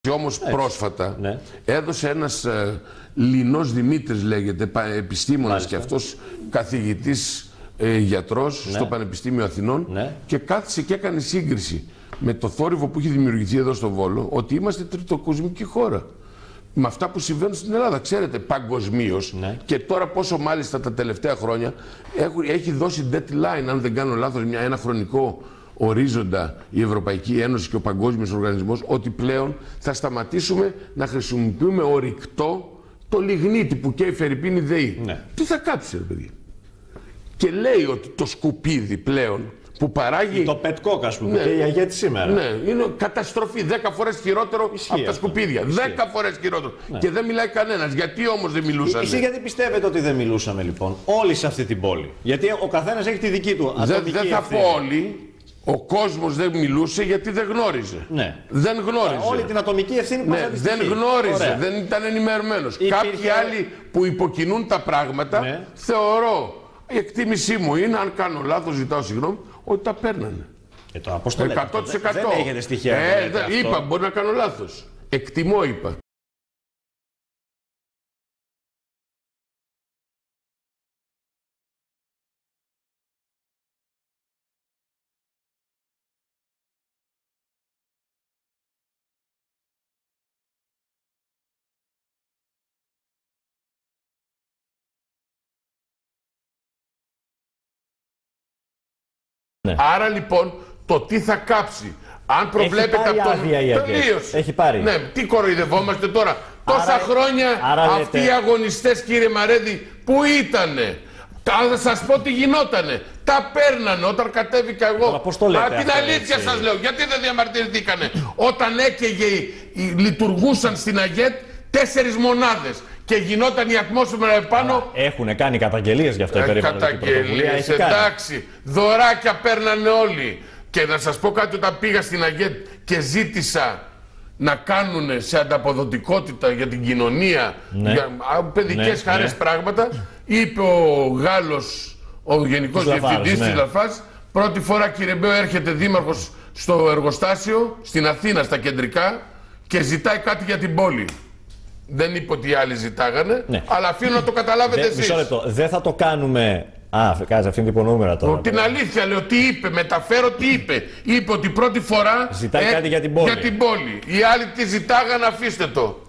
Καταγγελία Μπέου για χρηματισμό πολιτικών από την ΑΓΕΤ
Καταγγελία βόμβα ότι τα προηγούμενα χρόνια κάποιοι έπαιρναν «δωράκια» από την ΑΓΕΤ προκειμένου να μην υπάρχουν αντιδράσεις για την ρύπανση που προκαλεί έκανε μιλώντας την Παρασκευή στον τηλεοπτικό σταθμό Astra ο Δήμαρχος Βόλου Αχιλλέας Μπέος. Την ίδια καταγγελία μάλιστα είχε κάνει και προ ενός περίπου έτους αφήνοντας μάλιστα να εννοηθεί ότι μεταξύ αυτών που τα χρηματίζονταν ήταν και πρώην δήμαρχοι και πολιτικοί της περιοχής.